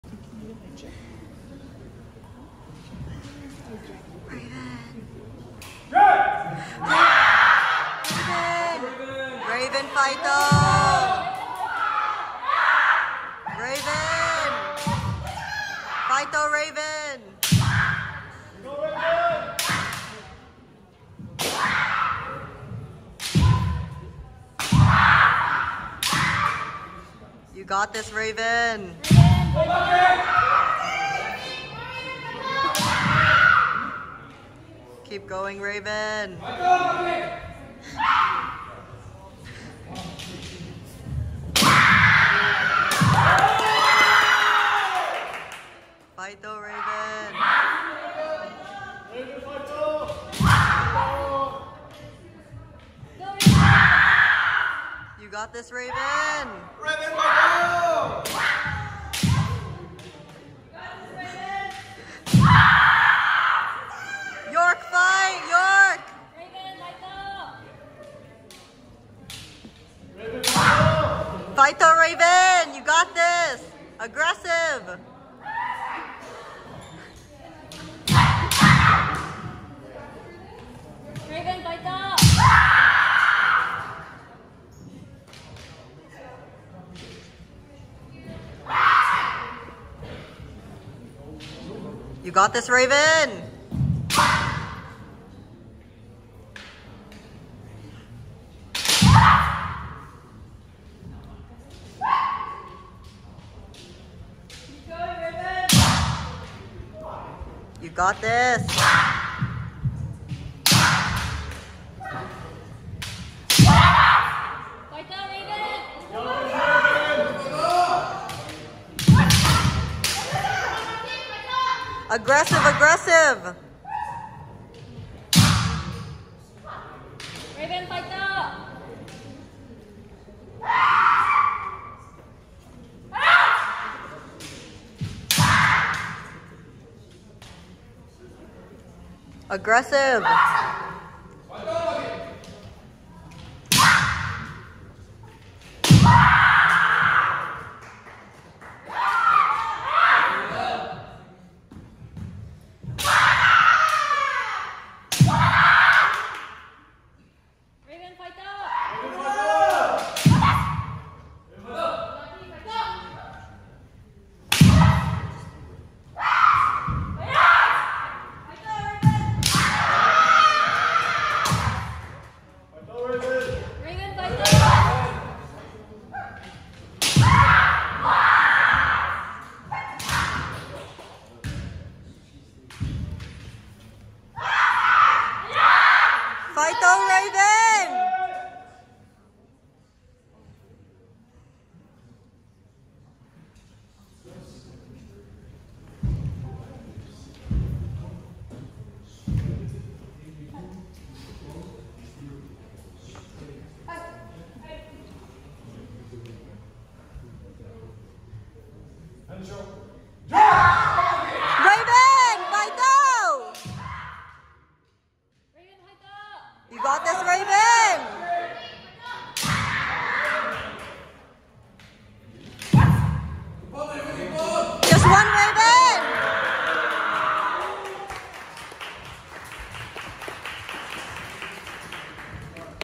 Women's Individuals R.Wong (YOR) vs M.Kamata (ETO) | JCCC 2023
Japanese Canadian Culture Centre Kendo 2023 tournament.